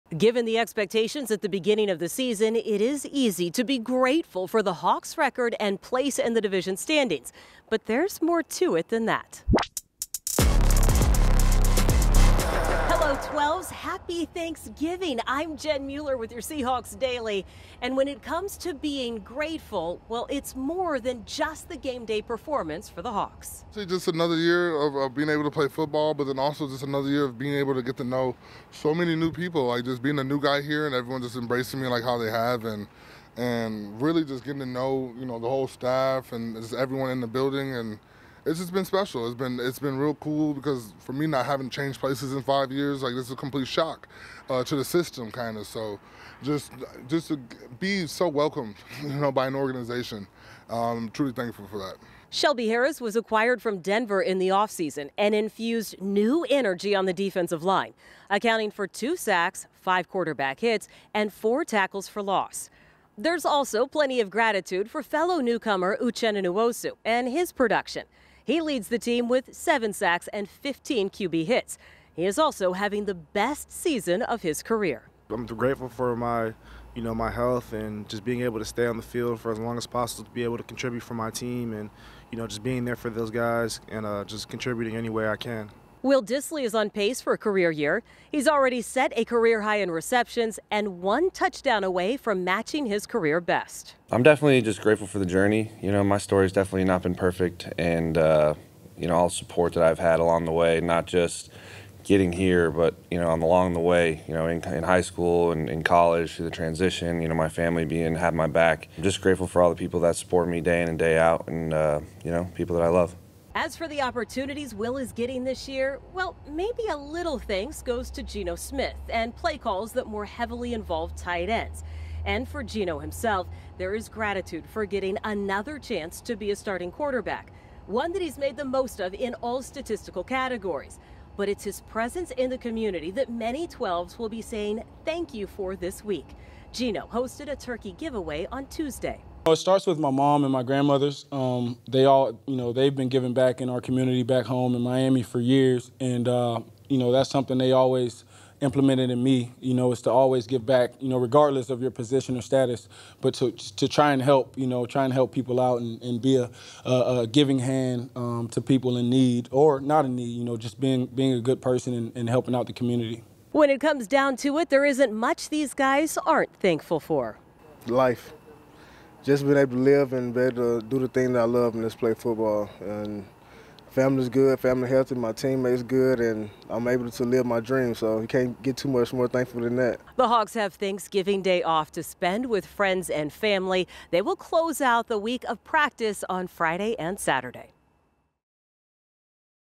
Seahawks Daily: Thankful
The Seahawks are thankful for the journey and appreciate where they're at both on and off the field this Thanksgiving.